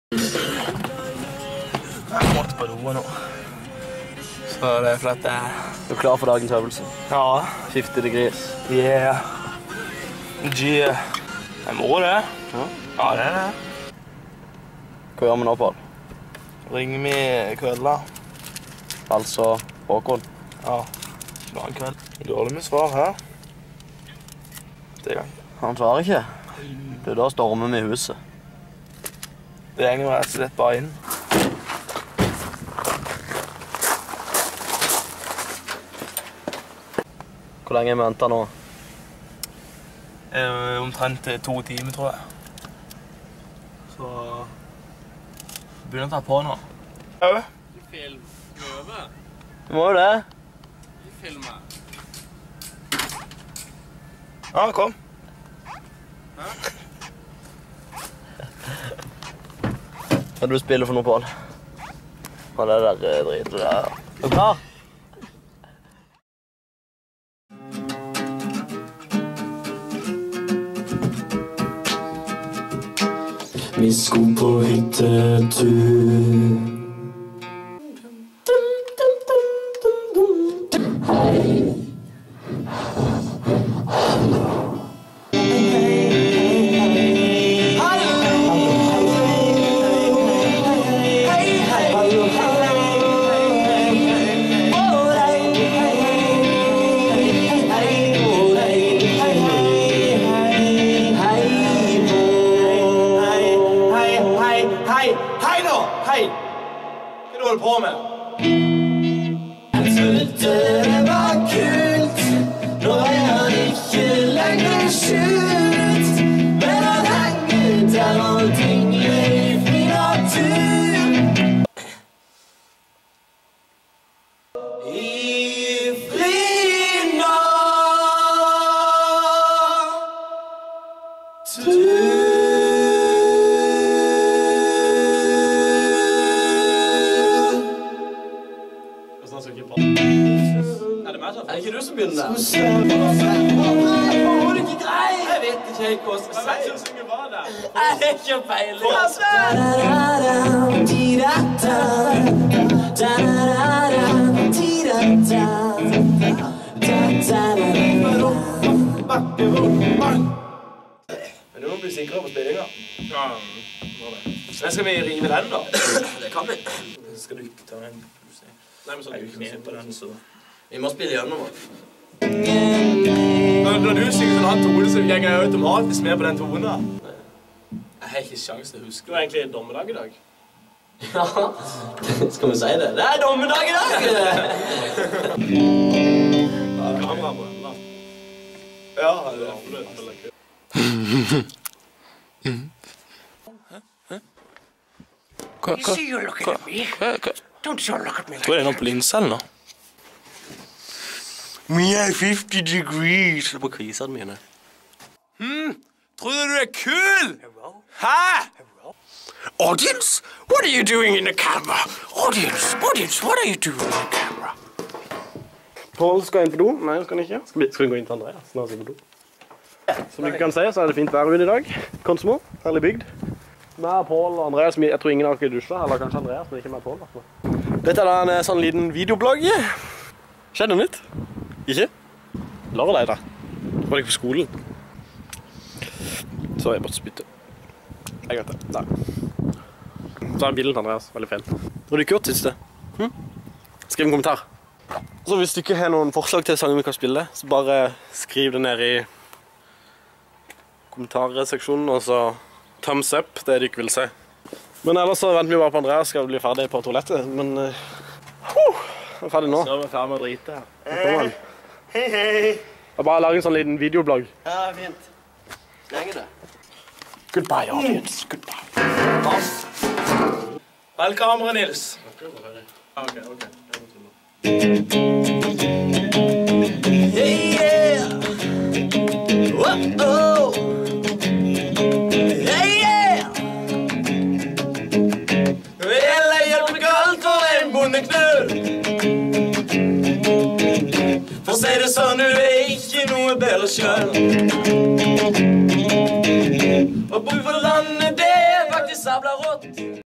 50 Degrees videoblogg (Mai, 2011)
Et lite innblikk i hvordan livet til 50 Degrees medlemmene er, og hvordan en normal øvelse kan utarte seg :)